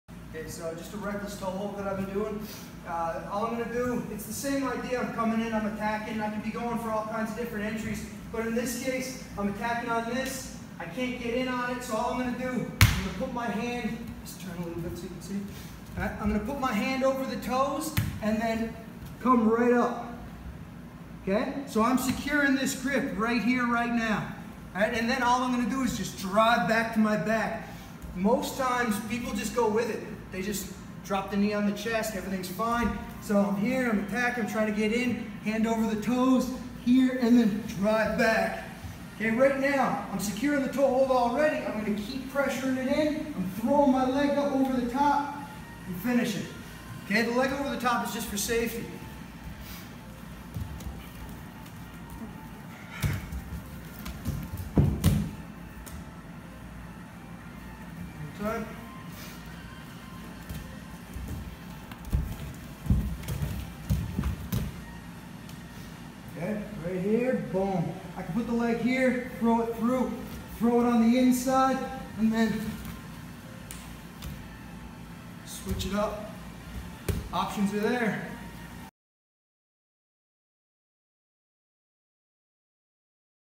Reckless Toehold From Seated Guard - ZombieProofBJJ (NoGi)
ZombieProofBJJ NoGi Techniques: This reckless toehold from seated guard (potentially turtle or failed shot) adds an element of scramble inducing surprise to your foot lock game 🔥 ... for more info on ZP, if you are in Cape Breton check us out for 30 DAYS FREE ... Text "FREE30" to NOW!!!

Coach Kent Peters
ZombieProof Martial Arts
Sydney, Nova Scotia, Canada